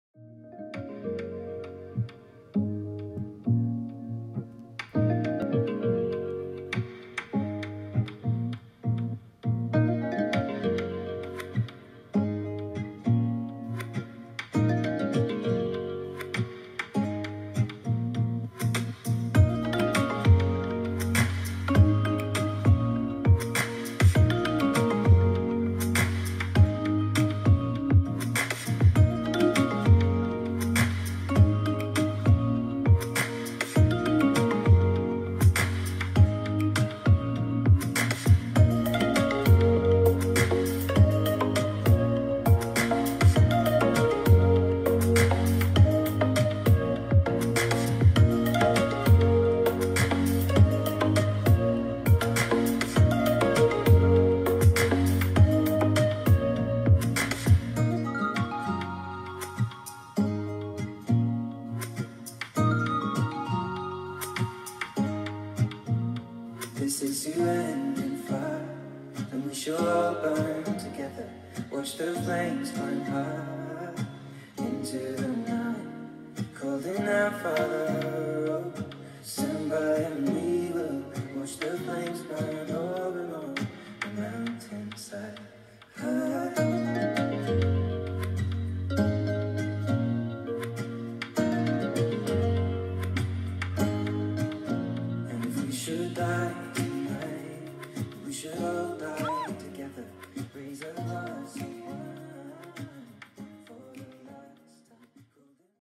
Challenges à Vélo/VTT en confinement! :) Bike Challenges during Lockdown! :)
Voici des idée de challenges dans votre jardin pendant le confinement!
Here are some new Challenges to do at home during the lockdown! :)